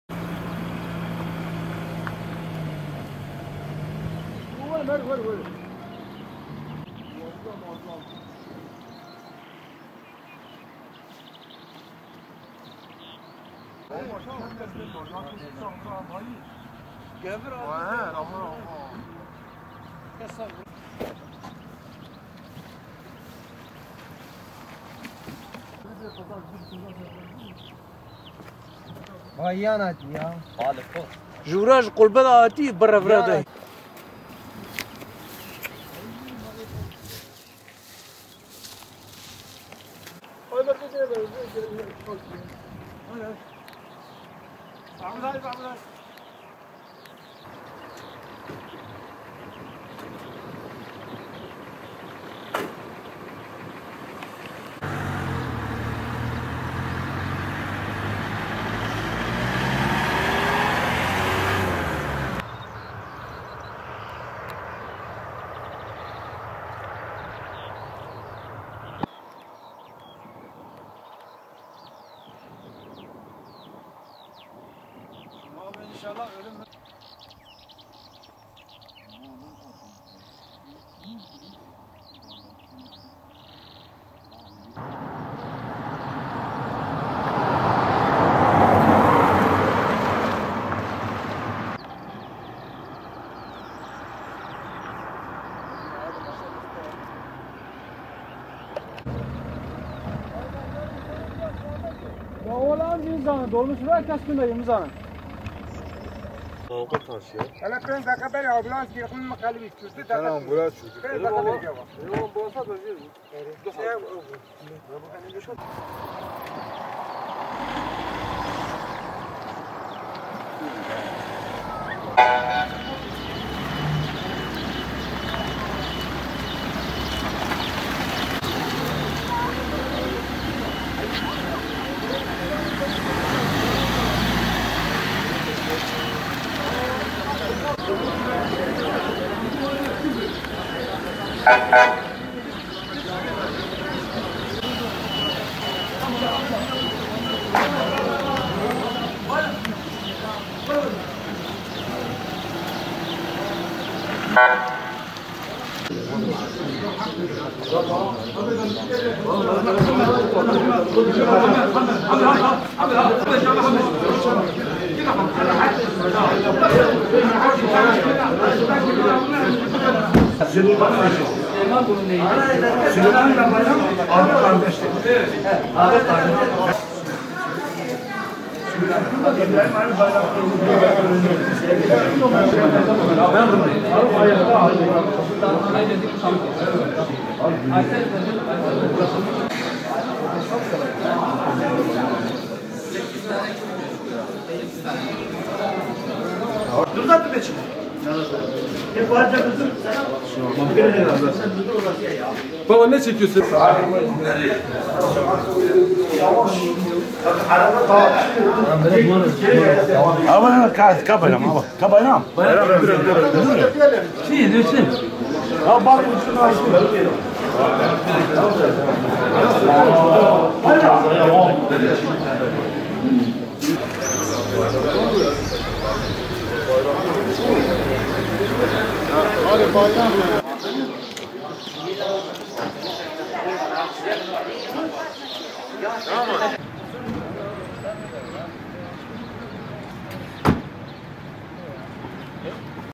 Yolcu minibüsü devrildi
Şanlıurfa'nın Siverek ilçesinde yolcu minibüsünün devrilmesi sonucu aynı aileden 8 kişi yaralandı.